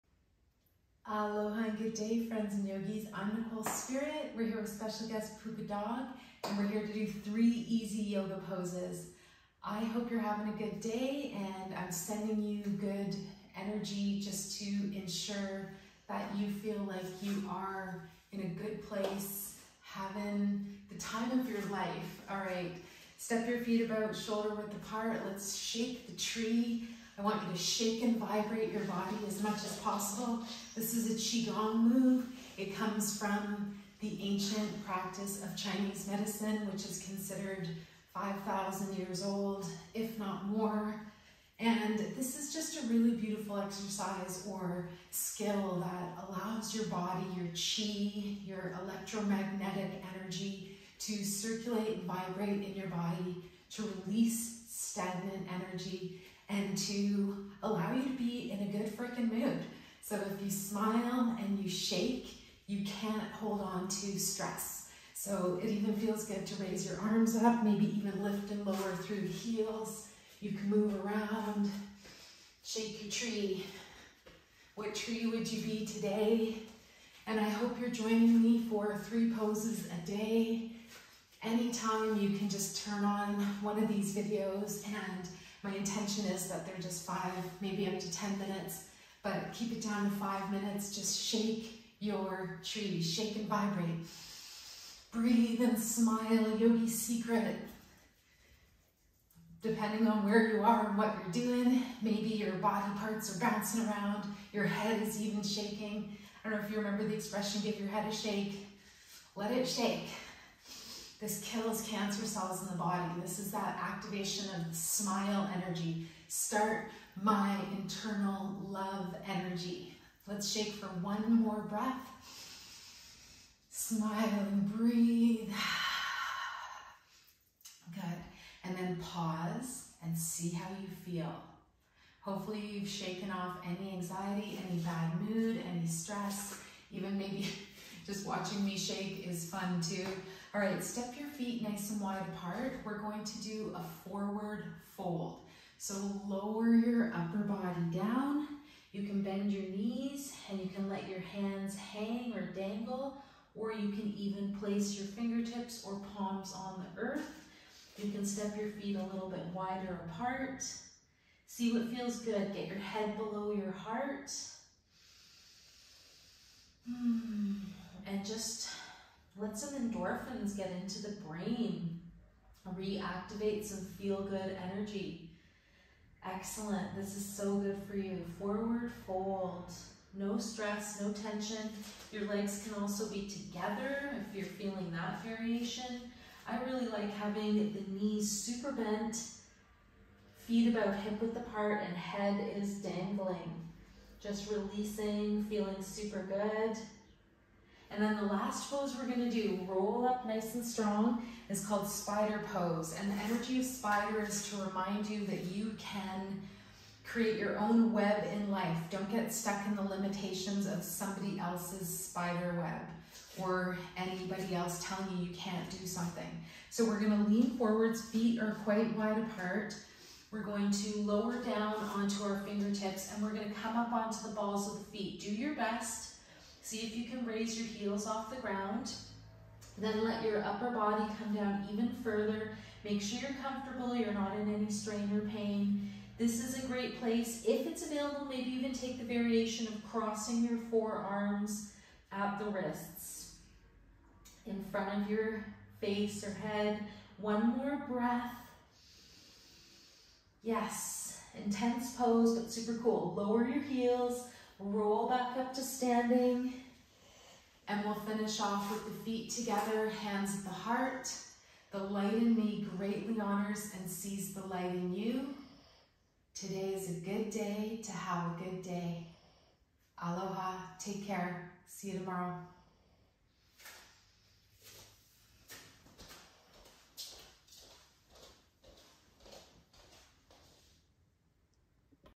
Weave Your Web - 3 easy yoga poses
Day 17/30 and we're doing 3 easy yoga poses a day. Join me, @yogawithnicolespirit and Puka dog to do less than 5 mins today. In this class we learn Spider pose and the wisdom it to make sure you weave your own web and don't get caught in the limitations of others. You're amazing!